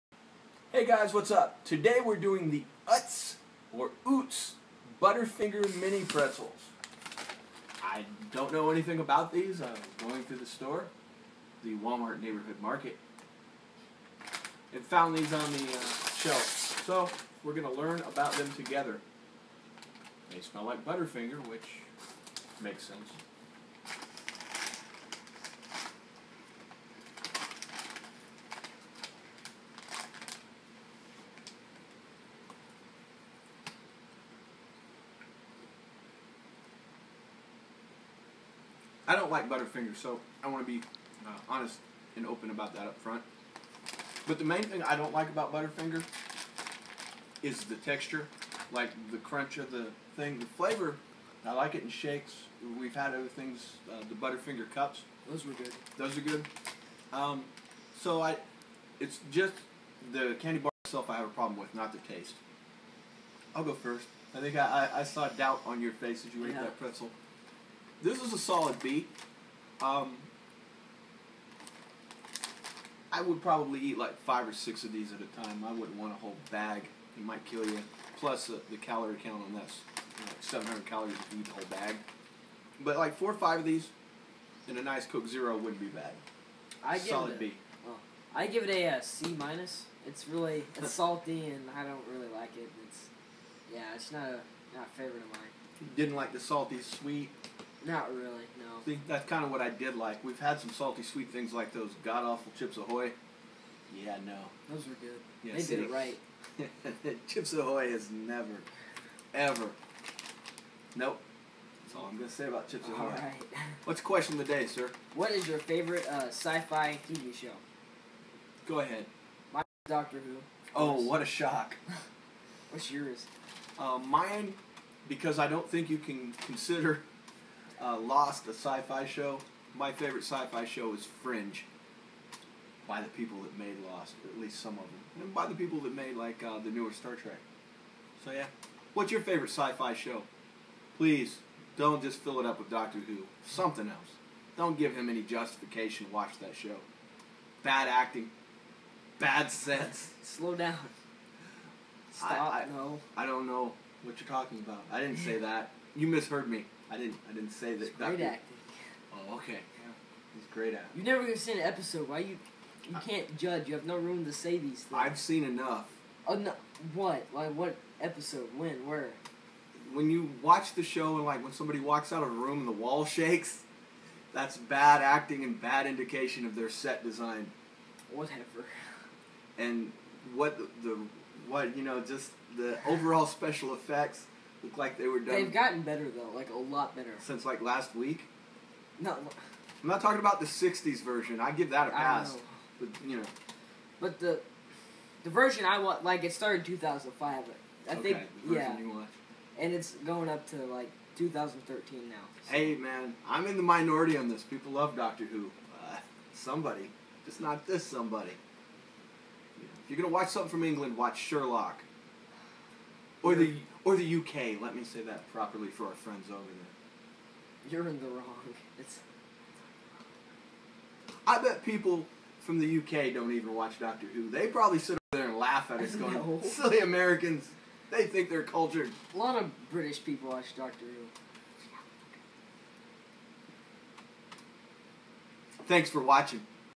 Utz Butterfinger Mini Pretzel Review QOTD favorite sci-fi show
today we review the Utz Butterfinger Mini Pretel. plus QOTD what is your favorite sci-fi show not syfy network show but actual sci-fi show but it can air on the syfy channel lol. come see us on Instagram .SEE Q ACCEPT THE ALS ICE BUCKET CHALLENGE